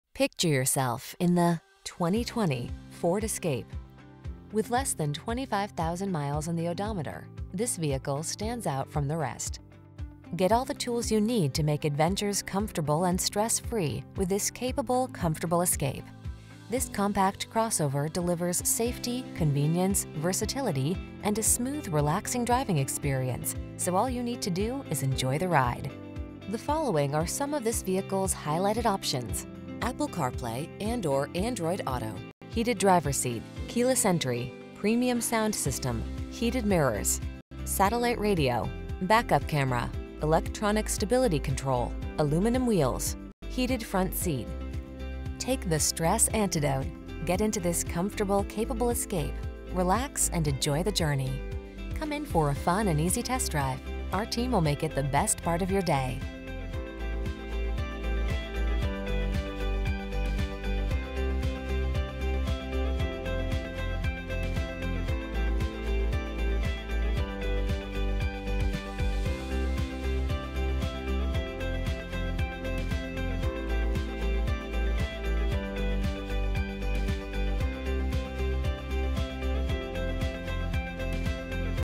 2020 Ford Escape Easton PA, Allentown PA , Bethlehem PA, Quakertown PA, Phillipsburg NJ FP12676
Ingot Silver Used 2020 Ford Escape SE available in Easton, Pennsylvania at Koch Ford. Servicing the Allentown PA , Bethlehem PA, Quakertown PA, Phillipsburg NJ area.

Koch Ford
3810 Hecktown Rd

CLEAN CARFAX... NO ACCIDENTS, ONE OWNER, INCLUDES PREPAID MAINTENANCE, AND KOCH 33'S EXCLUSIVE UNLIMITED TIME/100000 MILE POWERTRAIN WARRANTY, AWD, ABS brakes, AppLink/Apple CarPlay and Android Auto, Auto High-beam Headlights, Equipment Group 200A, Exterior Parking Camera Rear, Heated front seats, Heated Unique Cloth Front Bucket Seats, Low tire pressure warning, Power driver seat, Speed control, Steering wheel mounted audio controls. Odometer is 10270 miles below market average! Ingot Silver 2020 Ford Escape SE AWD 8-Speed Automatic 1.5L EcoBoostbrbr26/31 City/Highway MPGbrbrAwards:br  * JD Power Automotive Performance, Execution and Layout (APEAL) Study